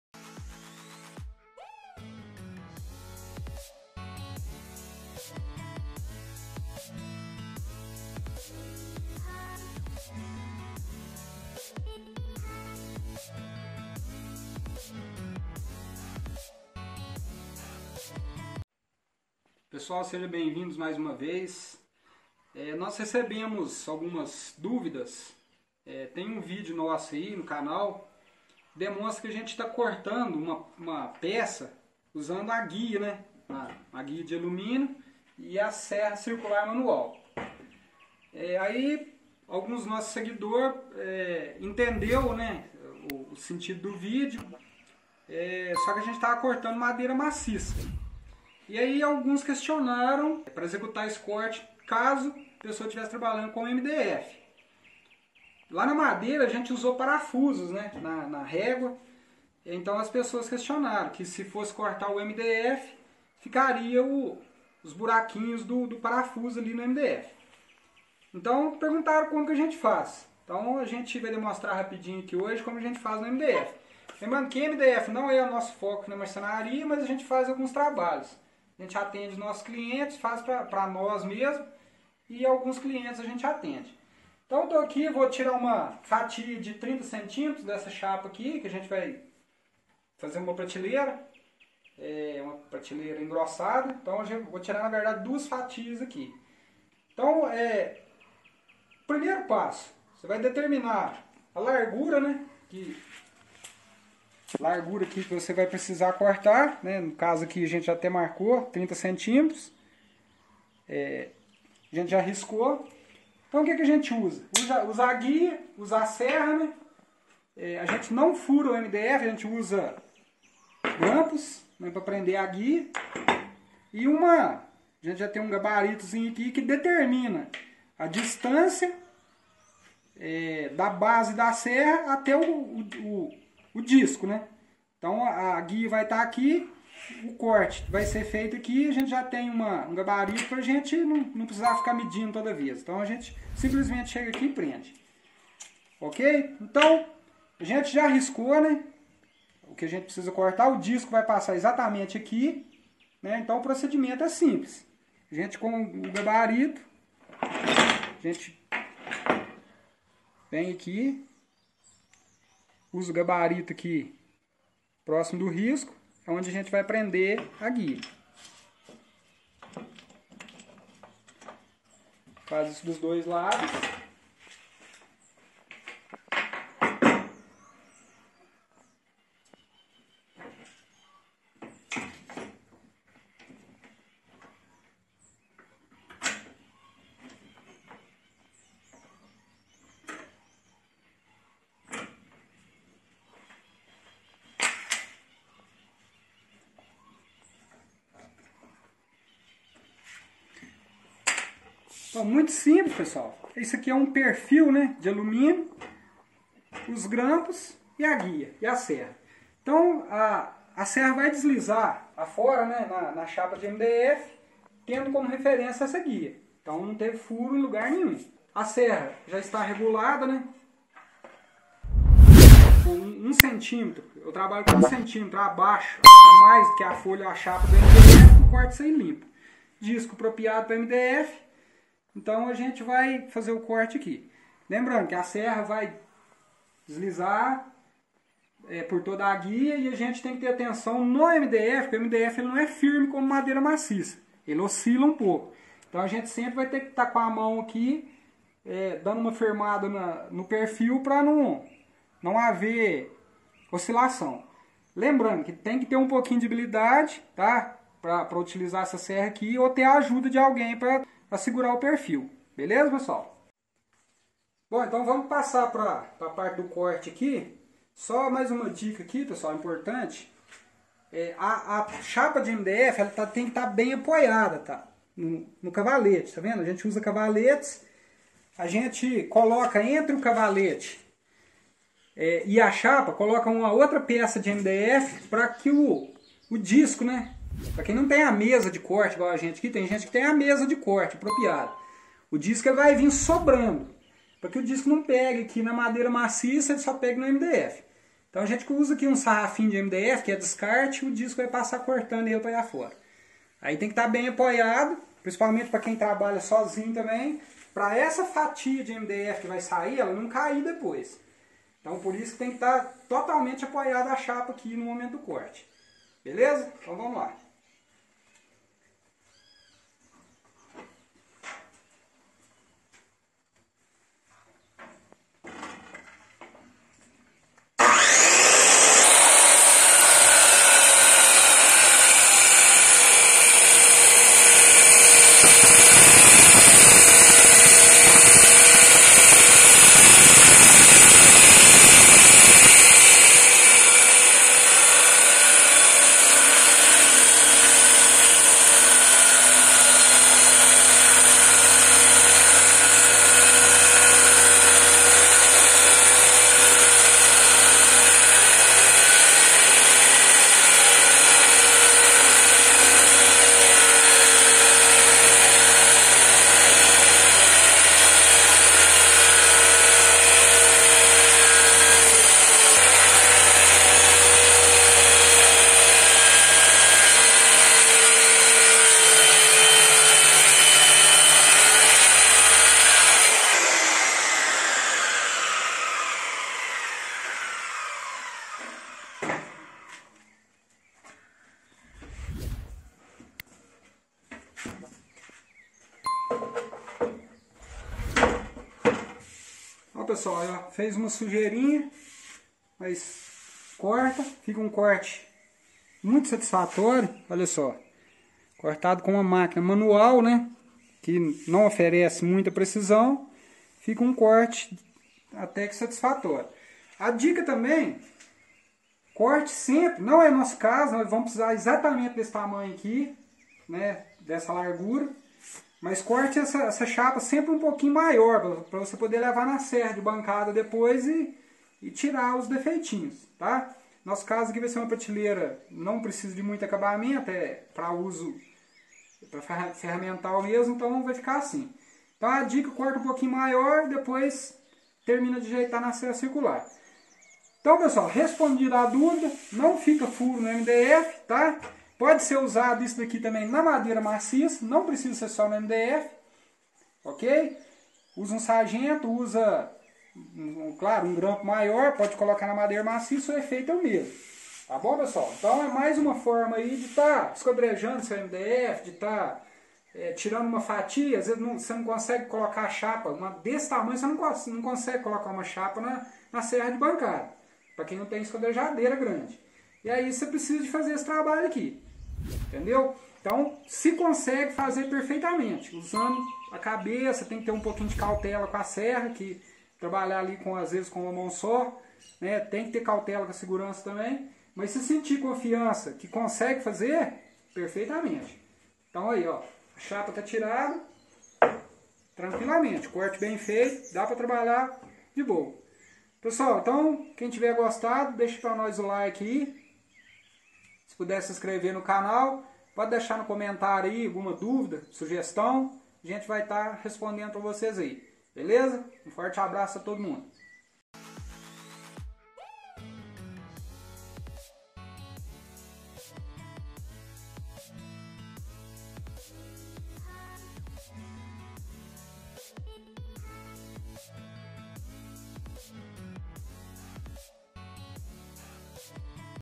Como cortar MDF usando serra circular manual e uma guia de aluminio de forma muito simples!
Apresentamos nesse video como cortar mdf de maneira muito simples, utilizando serra circular manual e uma guia de aluminio, presa com grampos.
  Apresentamos tambem a maneira como deve ser apoiado a chapa nos cavaletes para execução do corte com segurança , bem como a regulagem do disco da serra para que o corte fique com boa qualidade.
  Ferramentas nescessarias:
  Serra circula manual, 2 grampos, 1 guia de aluminio.
Abaixo segue links de videos relacionados a esse: